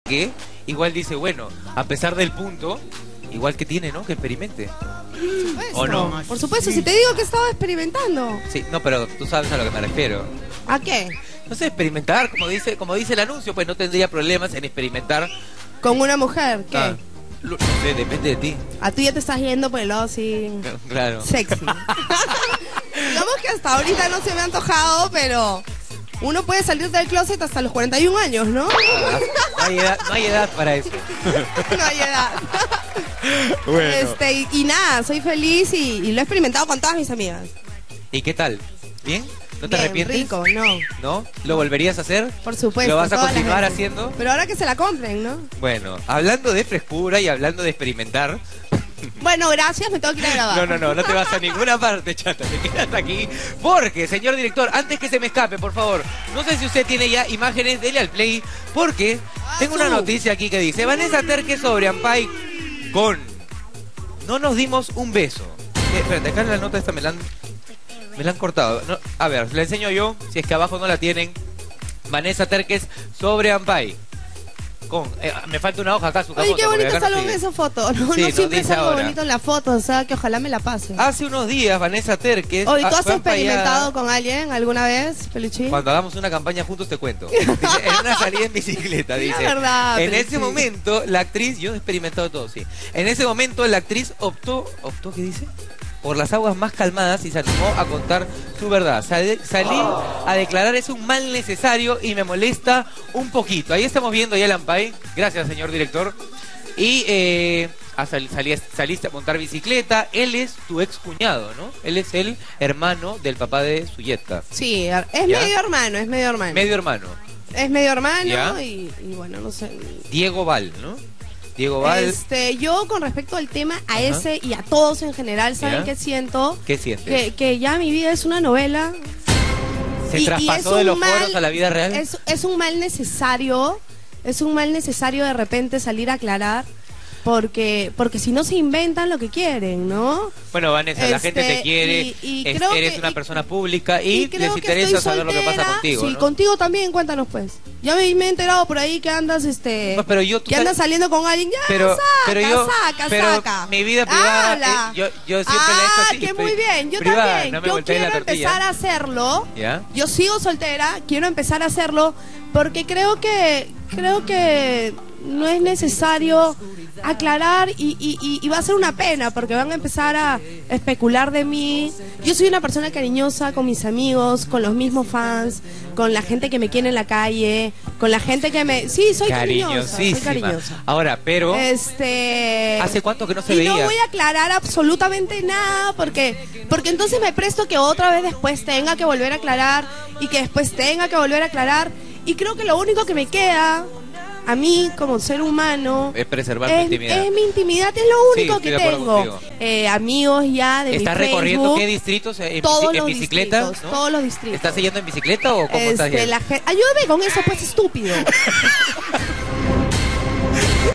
Vanessa Terkes:"No negare ni afirmare, una relación amorosa con Diego Val"
, Muy molesta, Terkes comentó que no negará ni afirmará si tiene una relación amorosa con Diego Val.